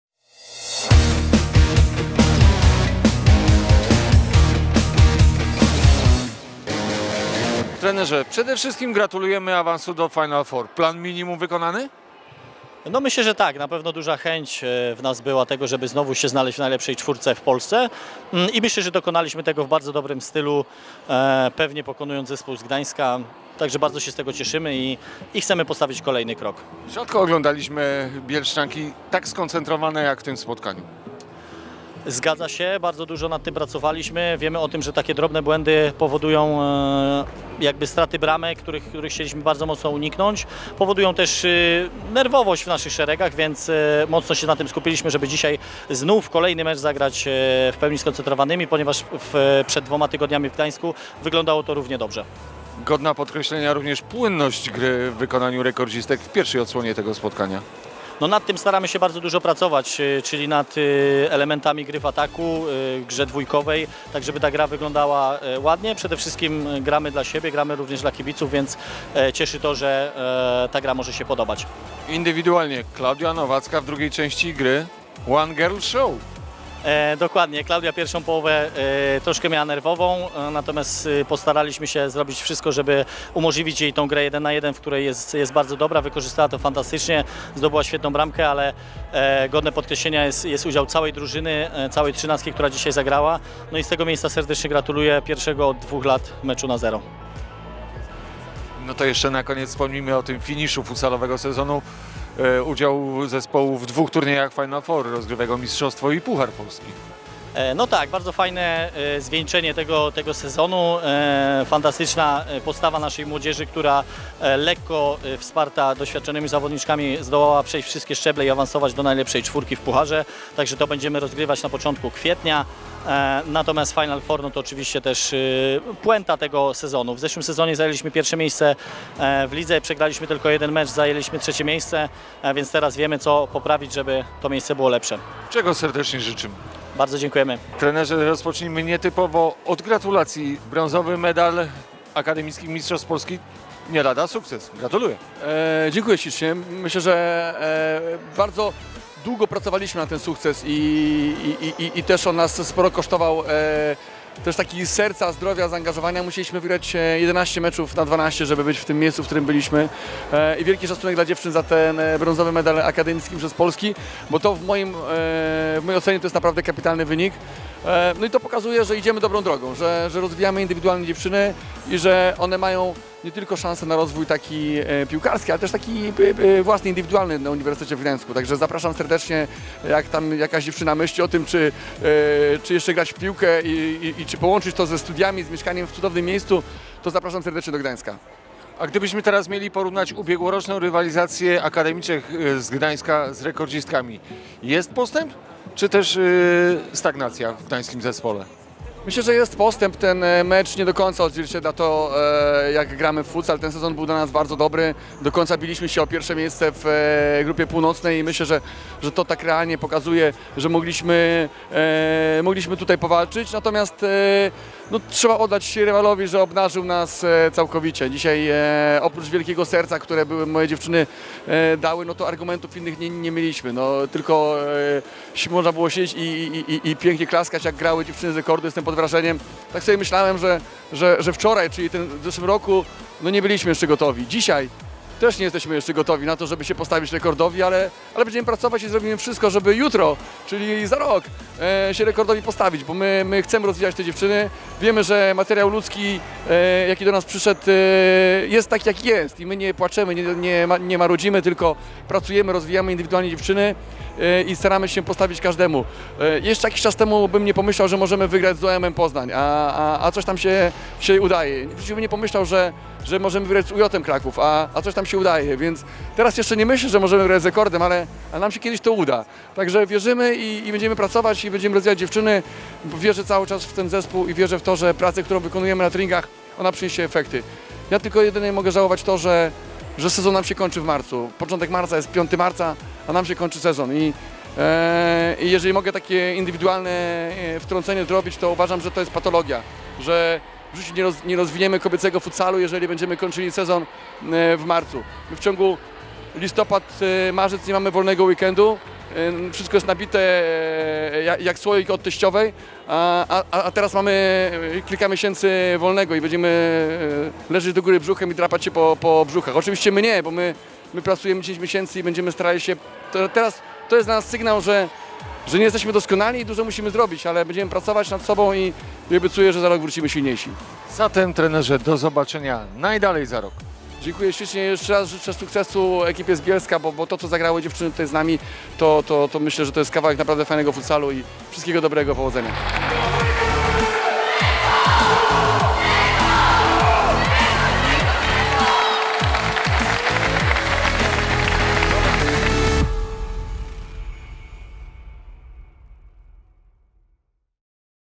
Rekord Bielsko-Biała – AZS UG Gdańsk 5:0 [SKRÓT I WYWIADY]
Rekord Bielsko-Biała – AZS UG Gdańsk 5:0 (2:0)
1:0 Chóras (3.min.)
2:0 Nowak (7. min.)
3:0 Nowacka (23. min.)
4:0 Czyż (23. min.)
5:0 Nowacka (28. min.)